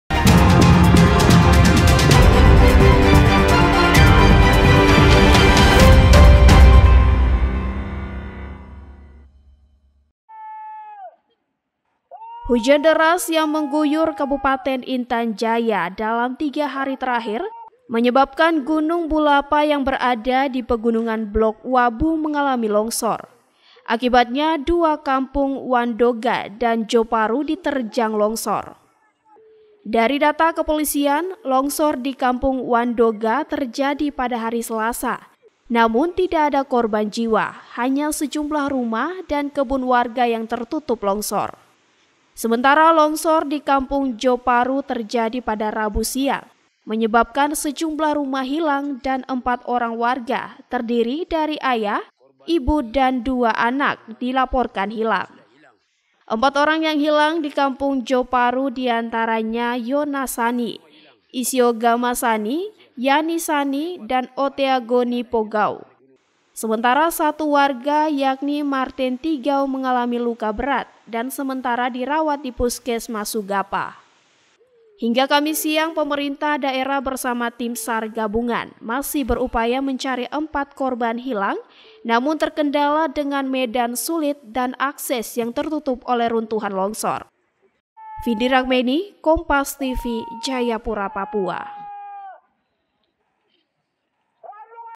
Tim SAR Masih Melakukan Pencarian Empat Orang Hilang Akibat Longsor
JAYAPURA, KOMPAS.TV - Hujan deras yang mengguyur Kabupaten Intan Jaya dalam tiga hari terakhir menyebabkan gunung Bulapa yang berada di Pegunungan Blok Wabu mengalami longsor. Akibatnya dua Kampung Wandoga dan Joparu diterjang longsor.

Dari data Kepolisian, longsor di Kampung Wandoga terjadi Selasa (06/02/2024), namun tidak ada korban jiwa, hanya sejumlah rumah dan kebun warga yang tertutup longsor.

Sementara longsor di Kampung Joparu terjadi, Rabu (07/02/2024), menyebabkan sejumlah rumah hilang dan empat orang warga terdiri dari ayah, ibu dan dua anak dilaporkan hilang.

Empat orang yang hilang di Kampung Joparu, diantaranya, Yonas Sani, Isiogama Sani, Yanis Sani, dan Oteagoni Pogau. Sementara satu warga yakni Marten Tigau mengalami luka berat dan sementara dirawat di Puskesmas Sugapa.

Hingga Kamis (08/02/2024), pemerintah daerah bersama tim SAR gabungan masih berupaya mencari empat korban hilang, namun terkendala dengan medan sulit dan akses yang tertutup oleh runtuhan longsor.